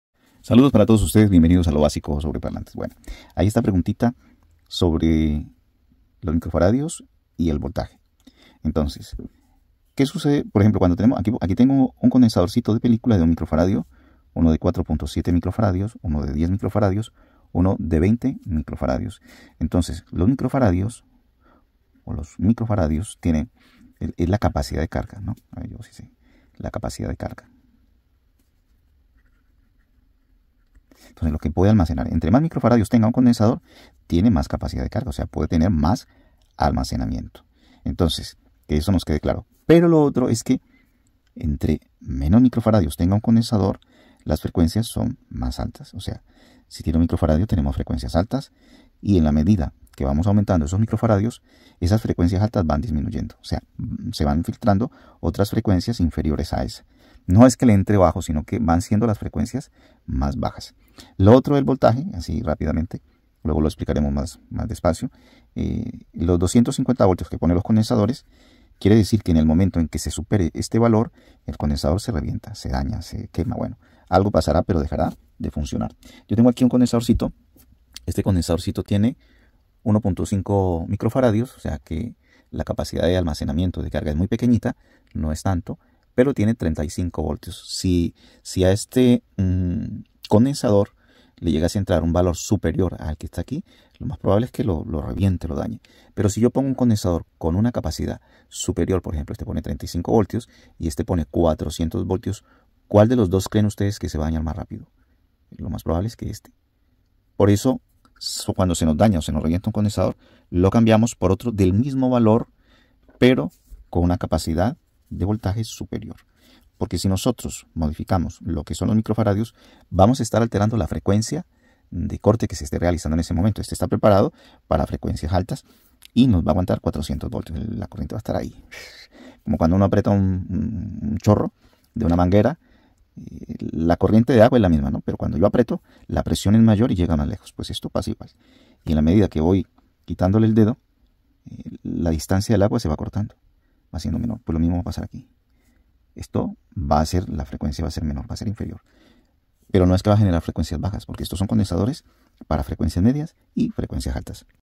Capacidad de Carga condensador para tweeter o parlante de medios
En el contexto de los parlantes, la capacidad de carga de un condensador no polar se refiere a su capacidad para almacenar carga eléctrica y suministrarla al circuito del parlante cuando sea necesario.

La capacidad de carga de un condensador no polar en un circuito de parlante depende de varios factores, como la capacidad nominal del condensador y la frecuencia de corte del filtro.

La capacidad nominal del condensador se mide en faradios (F) y determina la cantidad de carga eléctrica que puede almacenar.

En un circuito de parlante, el condensador no polar se carga y descarga continuamente a medida que se reproducen diferentes frecuencias.

La capacidad de carga del condensador debe ser suficiente para almacenar la carga necesaria y suministrarla al circuito del parlante sin distorsionar la señal de audio.

Es importante tener en cuenta que la capacidad de carga del condensador no polar debe ser adecuada para el circuito del parlante en términos de la frecuencia de corte y la impedancia del parlante.

Un condensador con una capacidad de carga insuficiente puede afectar negativamente la respuesta de frecuencia y la calidad del sonido.

En resumen: la capacidad de carga de un condensador no polar en un circuito de parlante se refiere a su capacidad para almacenar y suministrar carga eléctrica al circuito.

La capacidad de carga debe ser adecuada para el circuito del parlante en términos de la frecuencia de corte y la impedancia del parlante para garantizar un rendimiento óptimo del sistema de audio.